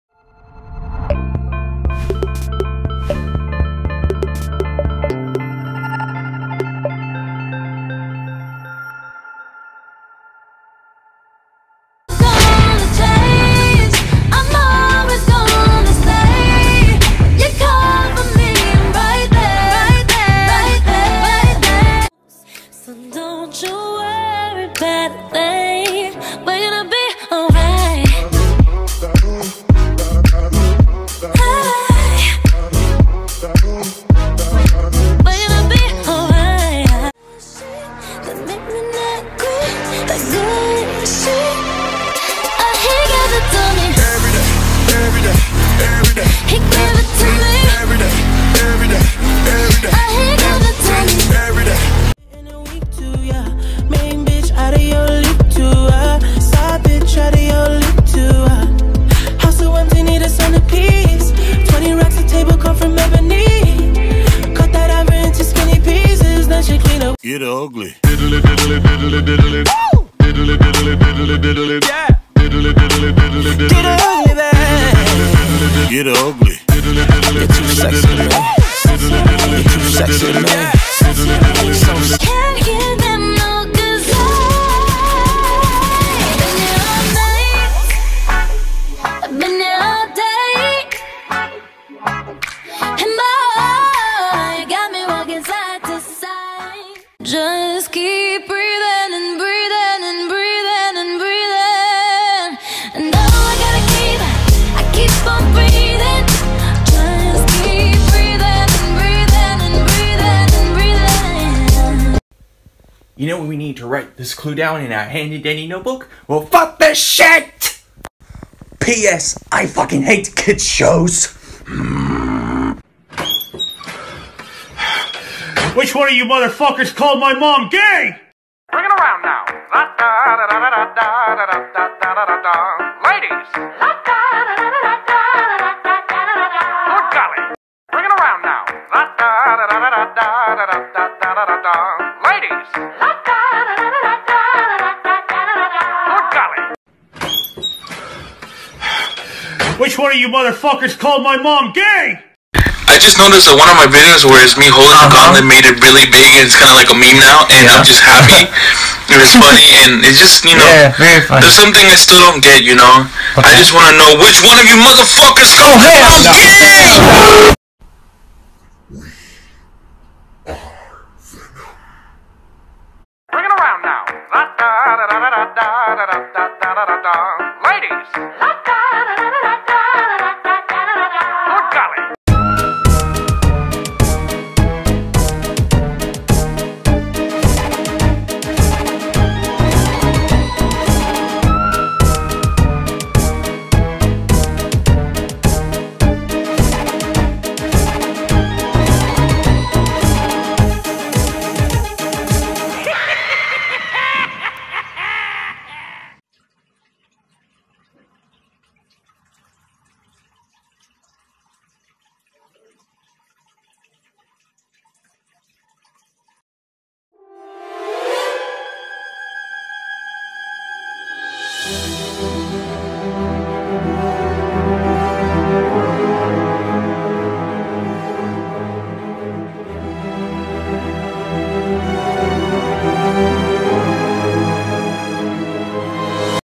CoolCorban95’s TikTok Compilation
I for those of you who don’t know, I made a new Compilation for my TikTok was formerly as musical.ly, I made a few posts on the app from 2017 or 2018 it is a bit funny but cringeworthy... don’t worry guys, I will still make more music, and more Nostalgia Chick fan-reviews will come to my channel as soon as the COVID-19 lockdown ends, see you guys soon.

WARNING:
This video is not made for kids 13 or younger